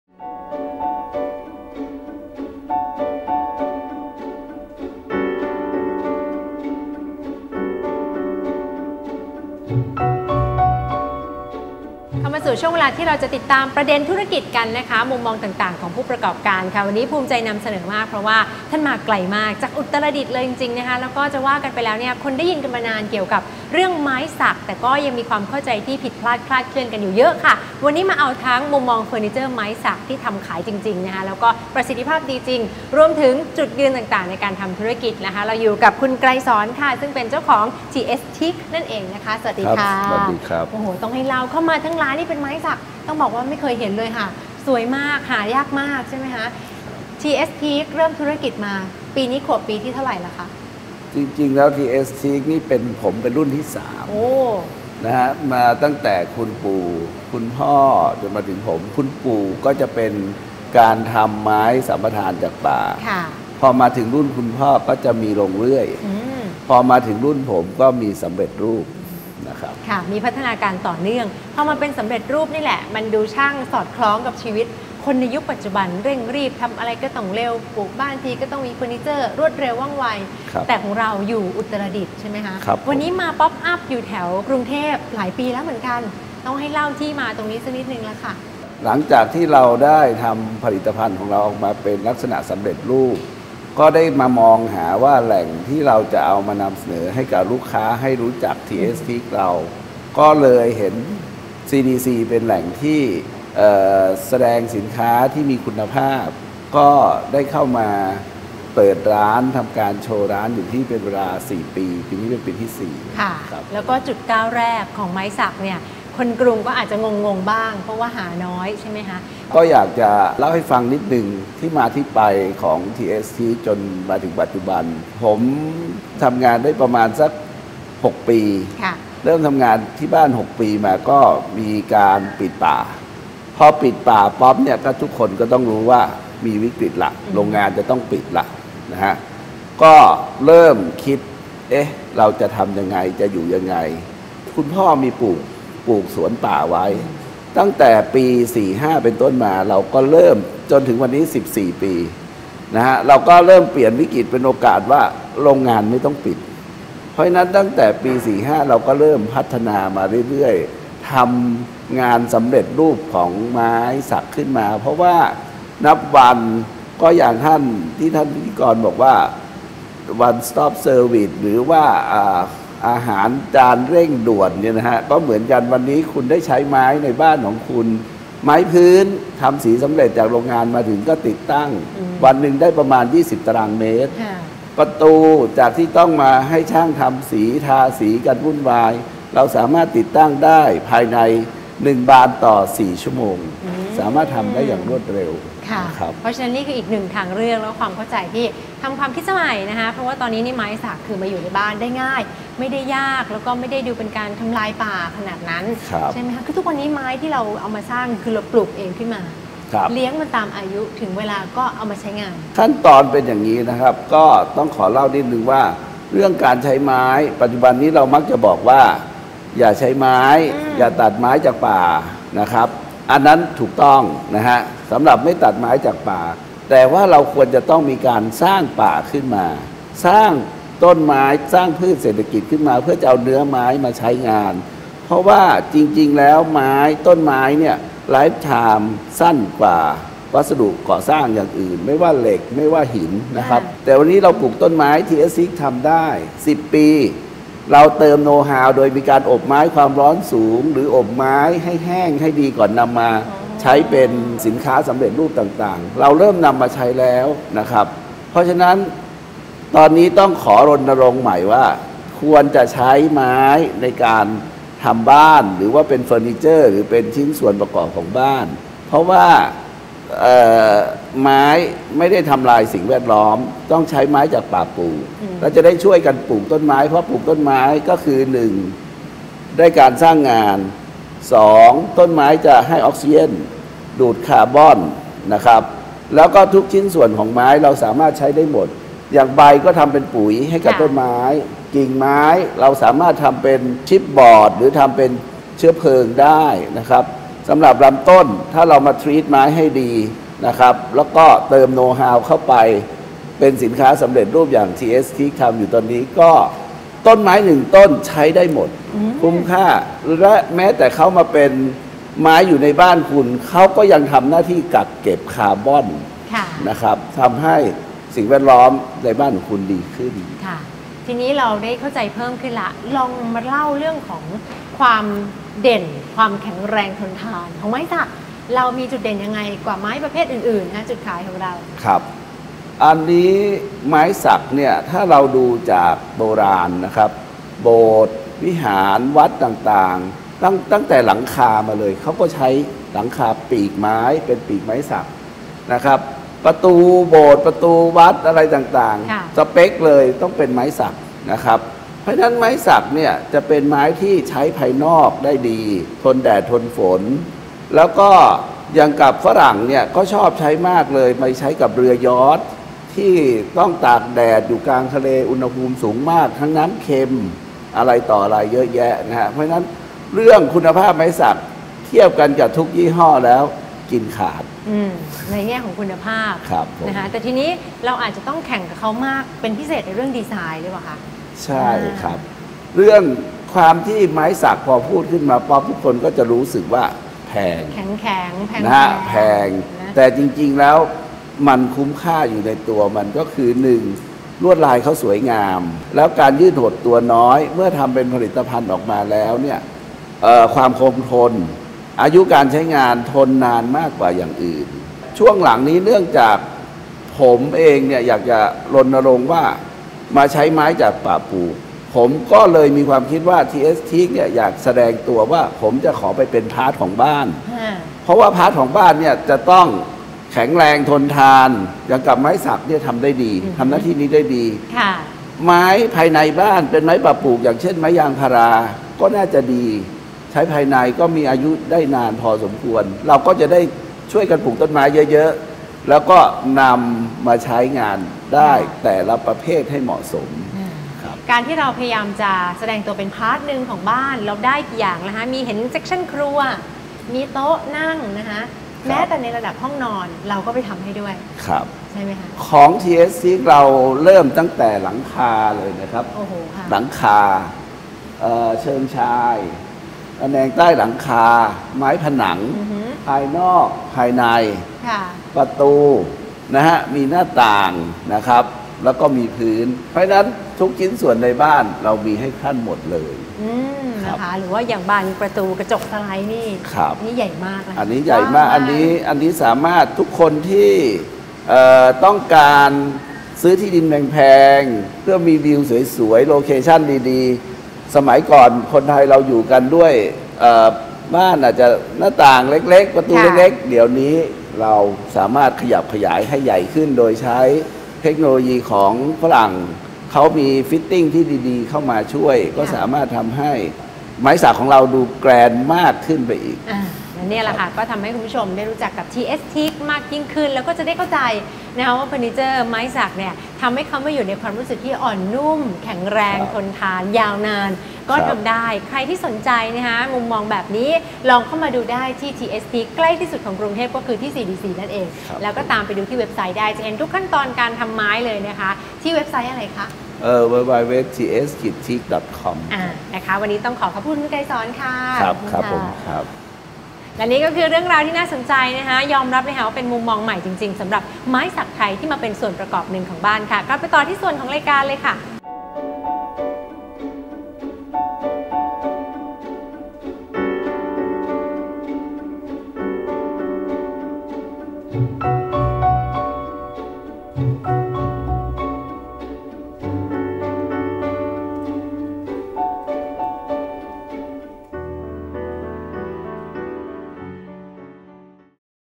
20150428 AEC TSteak
เฟอร์นิเจอร์ไม้สักที่หลายคนหลงใหล โดยไม่ทำลายสิ่งแวดล้อมจากการปลูกทดแทนตลอดเวลาอย่างเป็นระบบ ทำให้ ณ เวลานี้ “TS Teak” เป็นผู้ประกอบการเพียงรายเดียวในเมืองไทยที่ดำเนินธุรกิจเกี่ยวกับไม้สักครบวงจรตั้งแต่ต้นน้ำยันปลายน้ำเลยทีเดียว สนใจลองดูข้อมูลเพิ่มเติมได้ที่